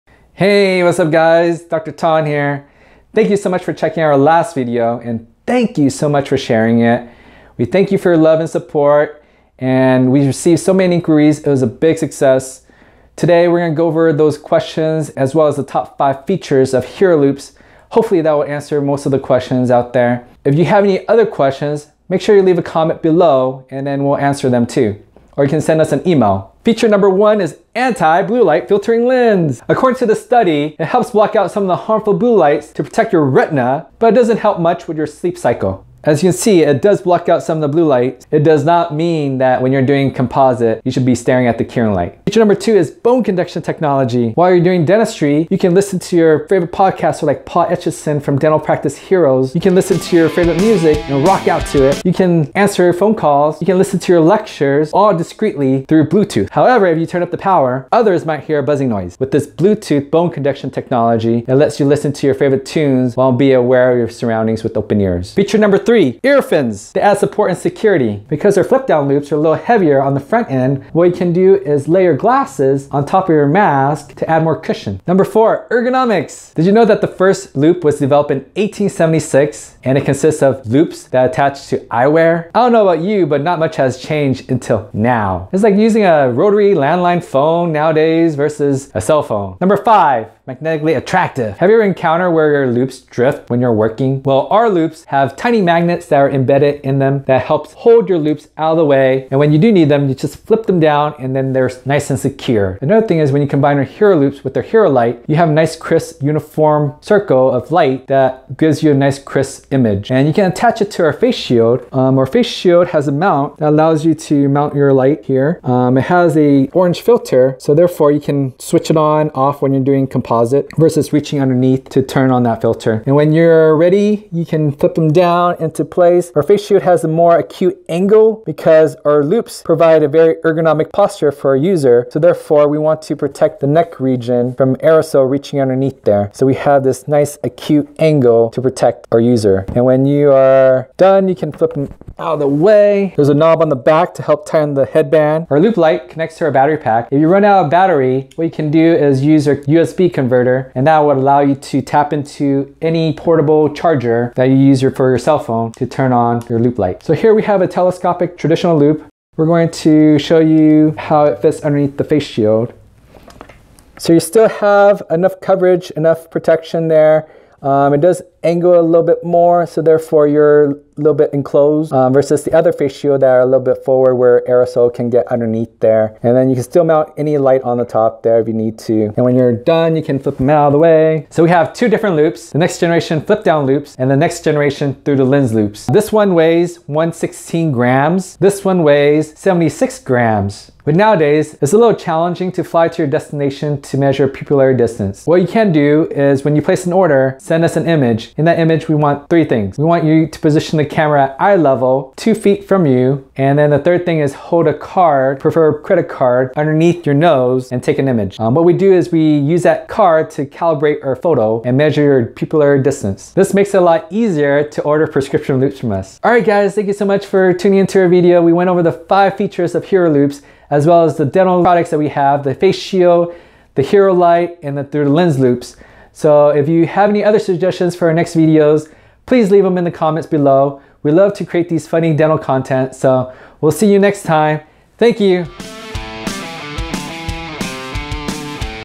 The Next Generation Dental Loupes - Top 5 Features - HERO LOUPES - Ergonomic Loupes - Ergo-Loupes
If you are in the market for a new pair of dental loupes, stay ahead of the pack and check out Hero Loupes Ergo-Loupes. They are the most ergonomic and affordable loupes. In this video, Dr. Trieu Ton will go over the top features of HL 2.5x and 3.5x, face shields, Hero Light and the next generation Thru The Lens. With their Patent Awarded product, Hero Loupes is changing the way dentists and hygienists practice dentistry, in a good way! Their motto is "See Differently" and geared towards helping Heroes protect their health and career.

Don't settle for ordinary loupes, get Ergo-Loupes!

Discussions on Hero Loupes, Lights, Face Shields, Thru the Lens, Flip Downs, How to Order, Anti Blue Light Filtering Lens, USB Converter, and many more!